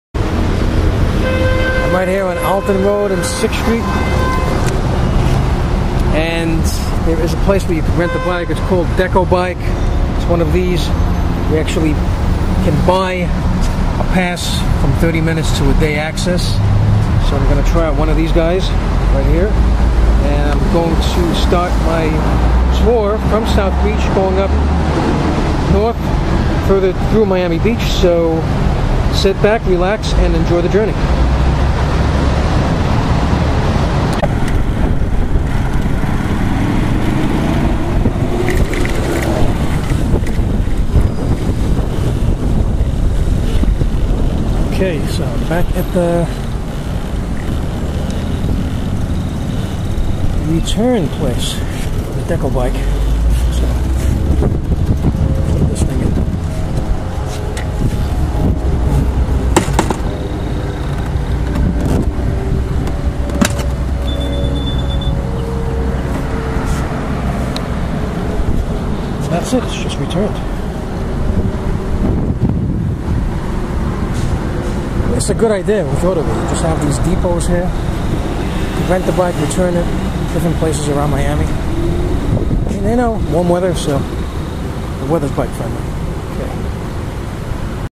Decobike Bicycle Rentals In Miami Beach FL Review
Decobike is a bicycle for rental company in Miami Beach FL. They have many different pickup / return places all over Miami Beach. I found it very convenient and cost effective. I used this bike to tour Miami Beach.